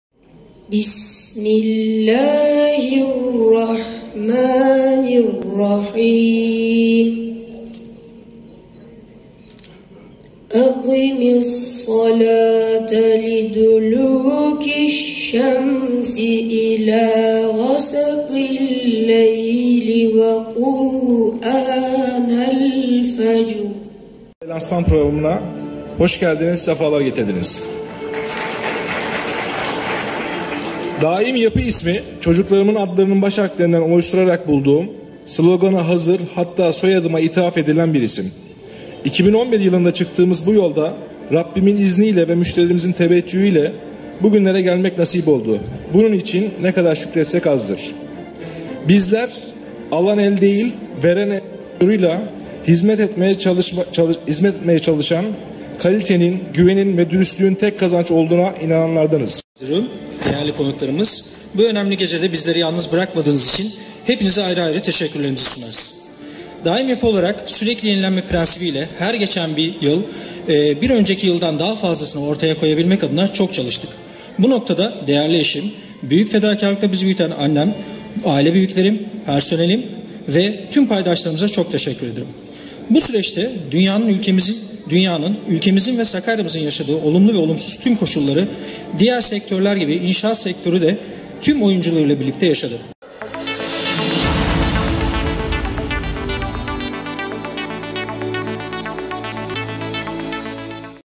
Daim Yapı 10'uncu yılını kutluyor
Sakarya’da gerçekleştirdiği önemli projelerle adını duyuran Daim Yapı, kuruluşunun 10’uncu yılını kutladı.